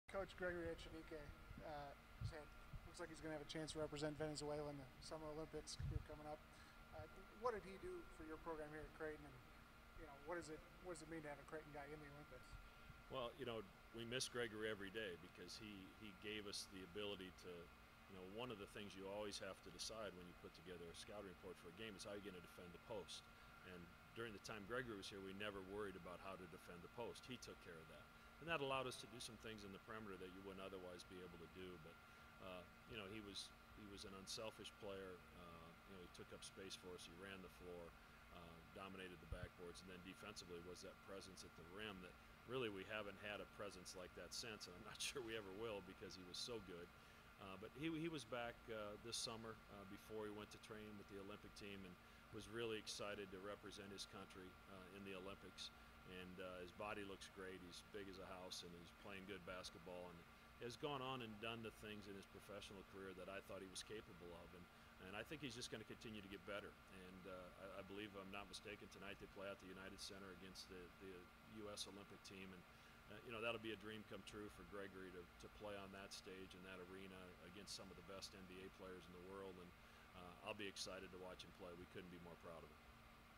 Creighton Coach Greg McDermott Discusses Olympian Gregory Echenique
Creighton head men's basketball coach Greg McDermott discusses former Bluejay center Gregory Echenique, who will compete in the 2016 Summer Olympics with the Venezuelan National Team.